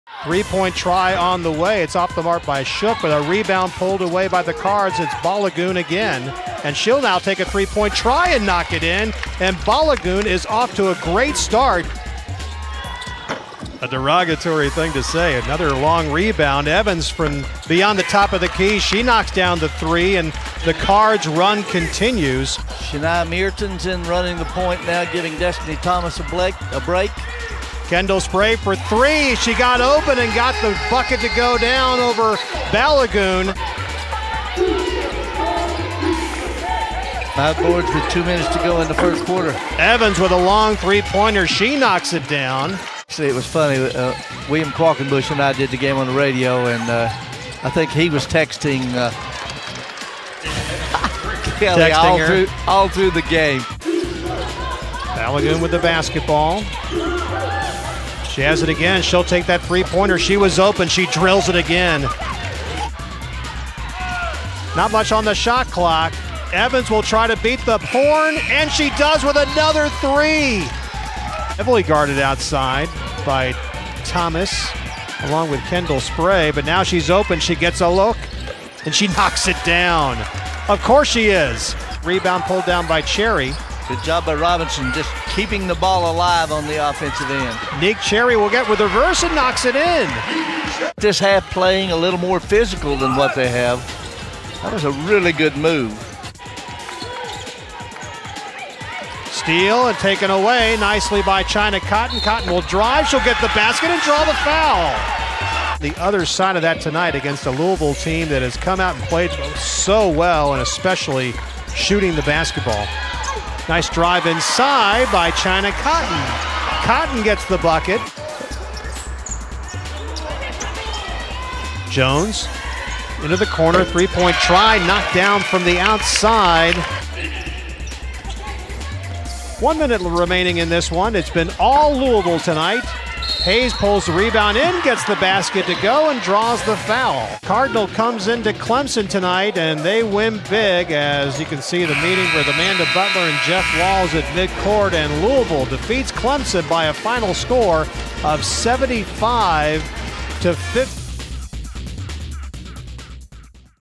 Louisville vs. Clemson Women's Basketball Highlights (2019-20)
Louisville vs. Clemson: The Louisville Cardinals built a big early lead at Clemson on Thursday and never looked back, winning 75-50.  Dana Evans made a career-high 7 three-pointers on her way to 27 points in the win.  It was the Cardinals' 5th straight win.